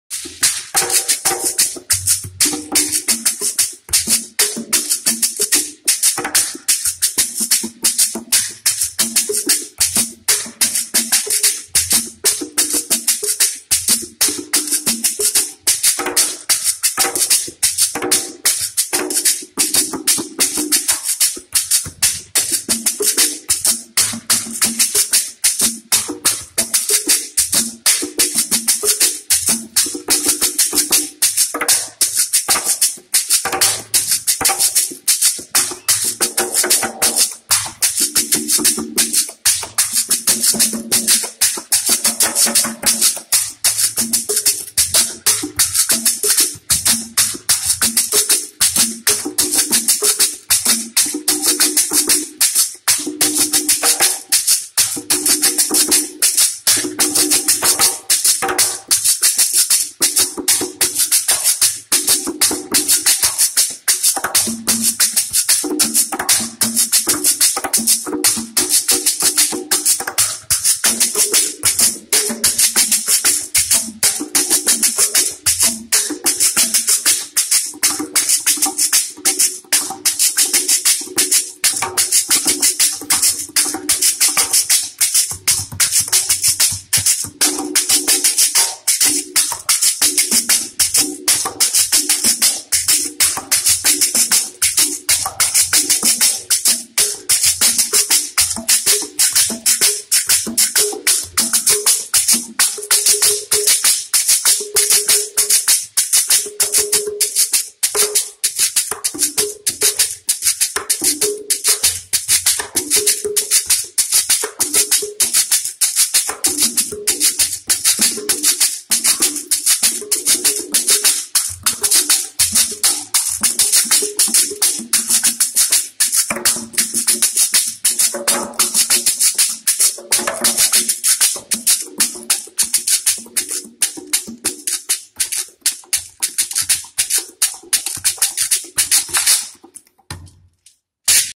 Anthony Carrillo and Jesus Andujar doing a little jam on DMD
Anthony Carrillo playing 4 different DMD Ashiko Cajons